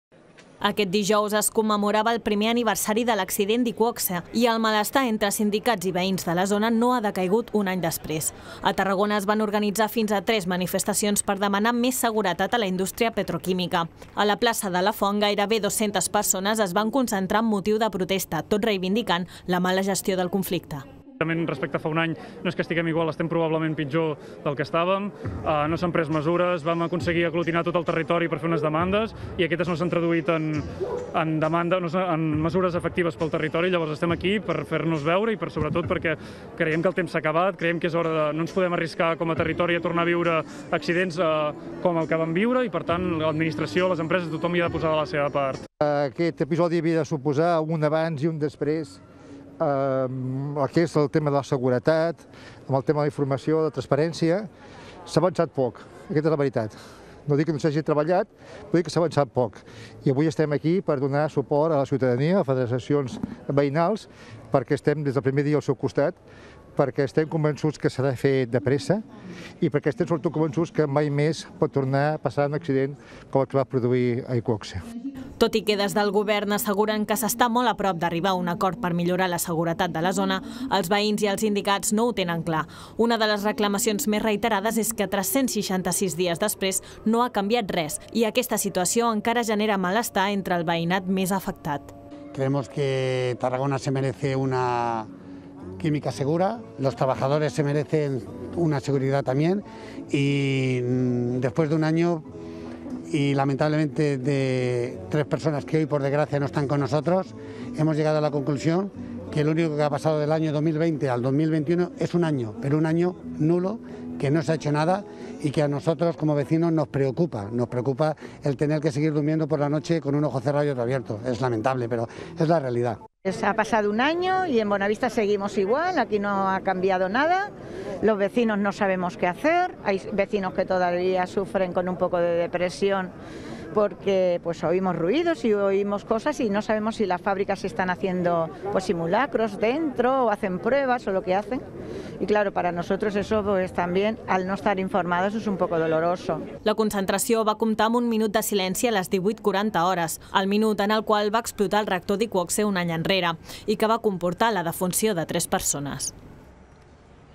Manifestacions a Tarragona un any després de l'explosió d'IQOXE
Sindicats i veïns de la zona han convocar diverses manifestacions per reivindicar la manca de seguretat un any després de l'accident, ja que, segons diuen, no ha canviat res.